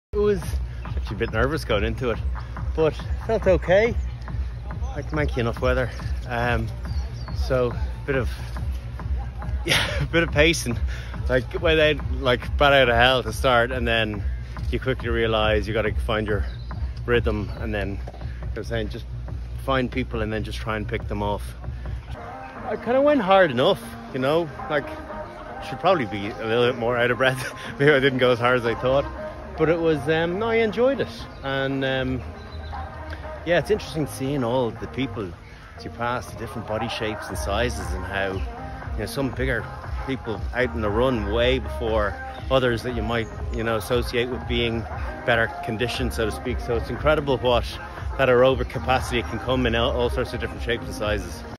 Irish rugby legend Brian O'Driscoll talks triathlon
Brian O'Driscoll completed the bike leg as part of a relay team with his Off The Ball crew at TriAthy 2022.